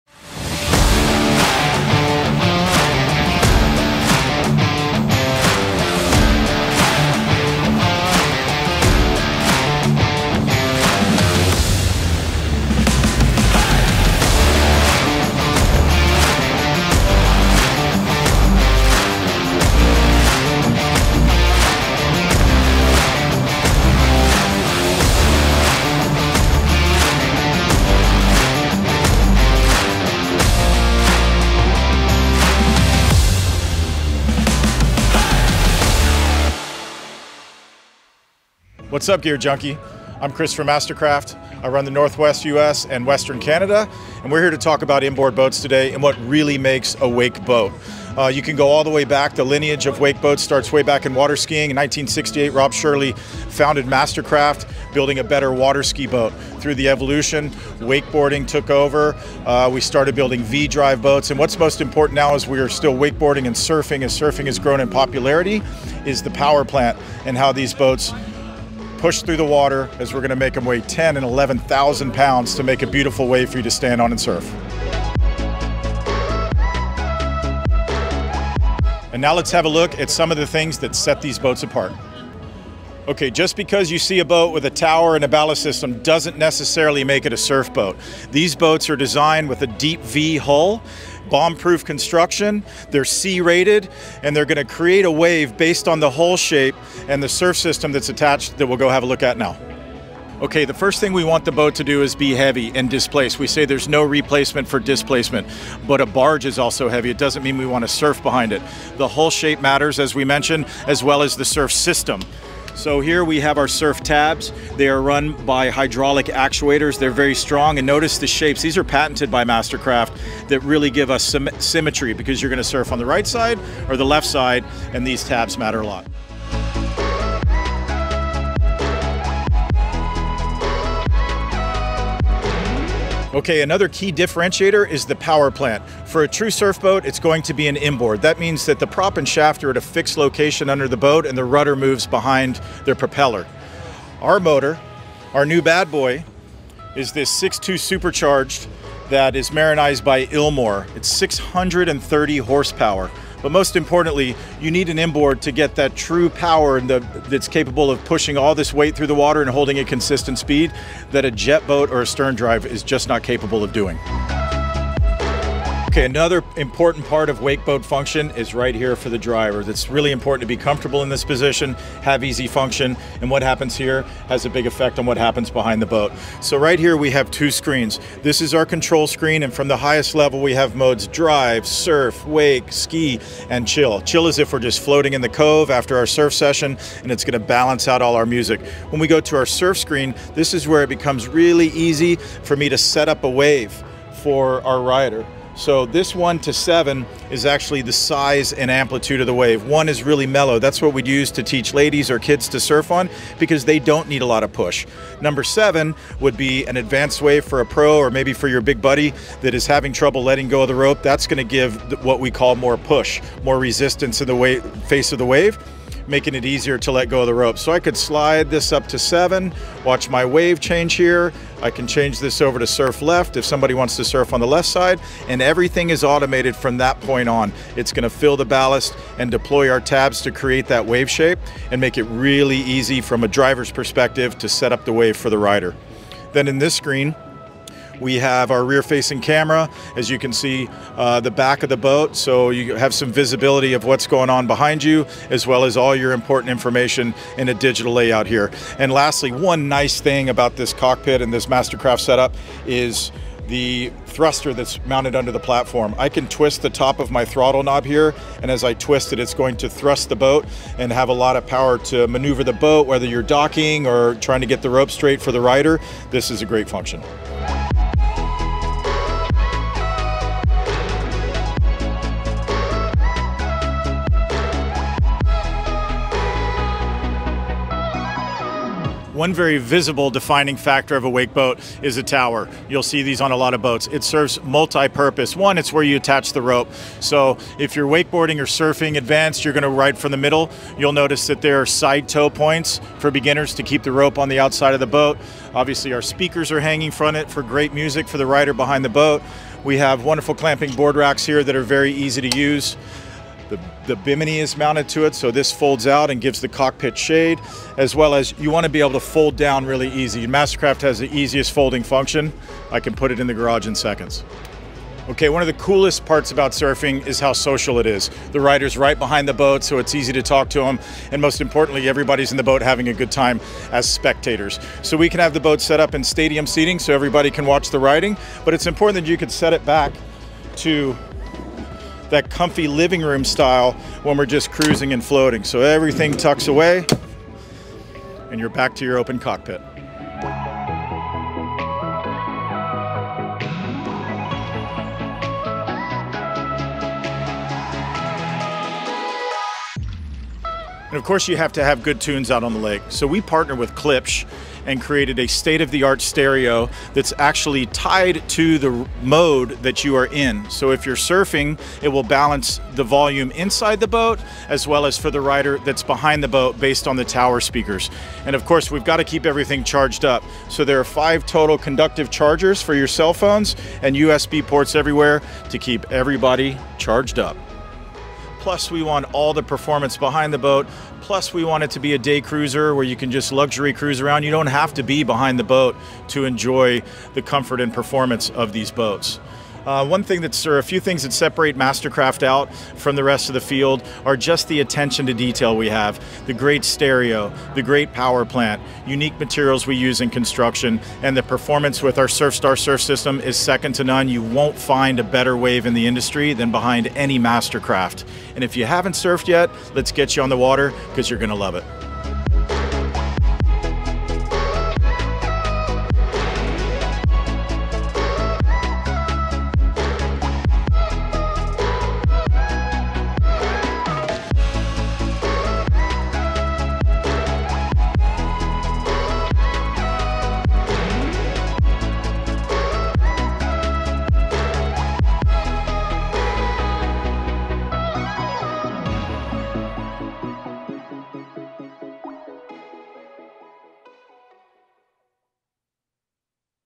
Inboard Wake Boats Explained with MasterCraft
A look at what makes modern wake boats the stoke machines that they are.

We caught up with Chris Sullivan, MasterCraft Sales Rep. for the NW U.S./Western Canada, at the Discover Boating Miami International Boat Show. He took us through the MasterCraft X24 and discussed the differentiators between an inboard wake boat and other tow boats on the market.

Then we got on the water with some professionals: Austin Keen and Parks Bonifay. They did their thing to showcase the technology in action.